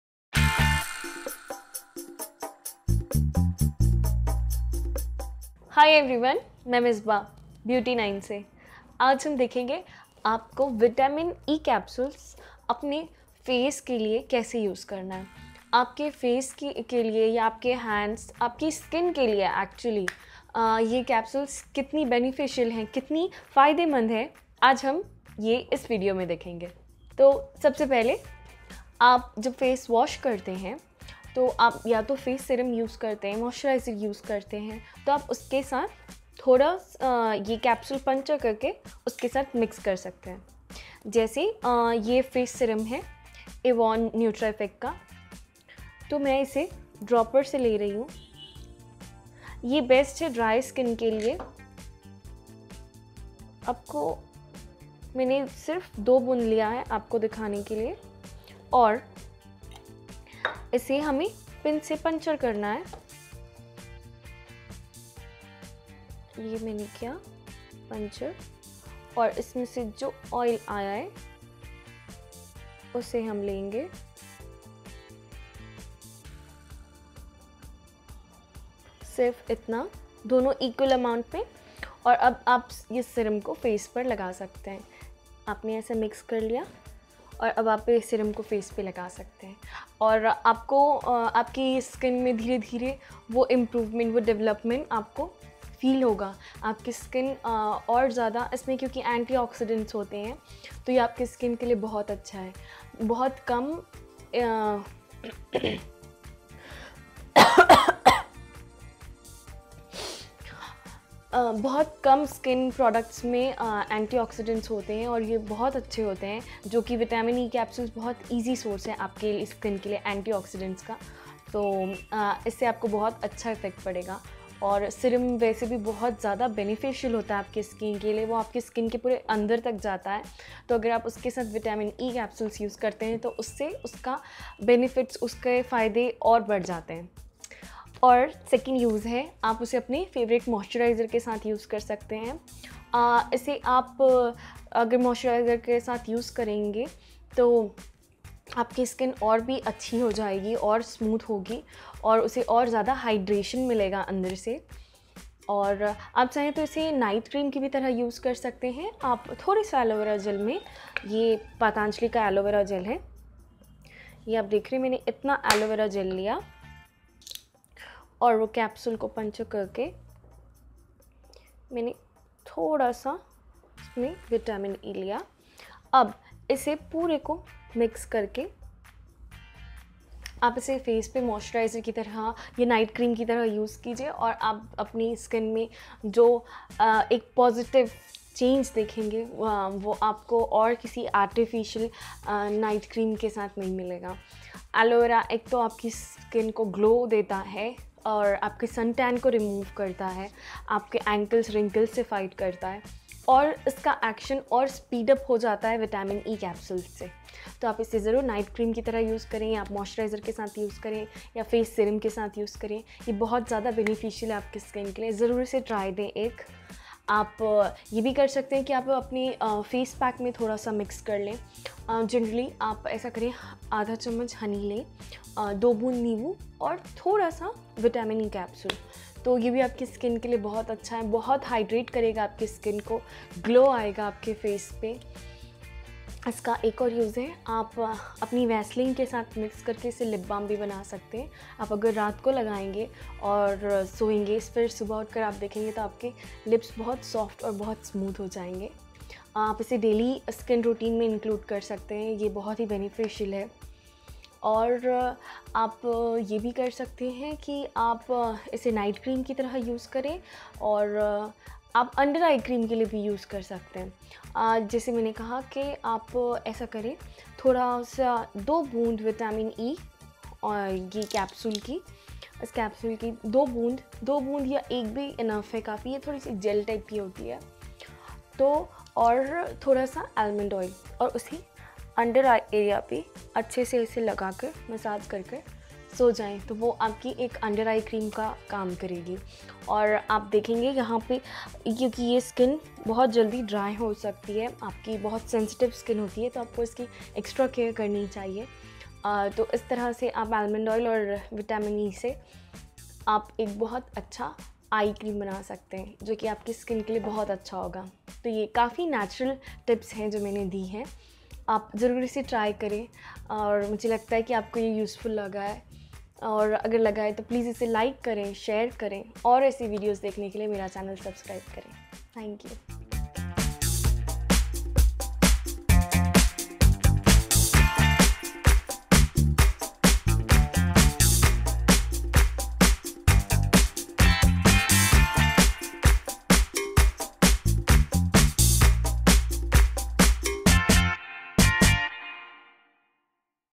How to Apply Vitamin E Capsule on Face (Hindi)
Learn how to apply vitamin E capsule on face or skin in this step by step tutorial from Misbah.

Products Used (with Amazon Links):
Genone E-Gen 400 Vitamin E Capsule
Avon Nutra Effects Facial Skin Serum
NutriGlow Aloe Vera Massage Gel
Dabur Badam Oil
Dabur Badam Oil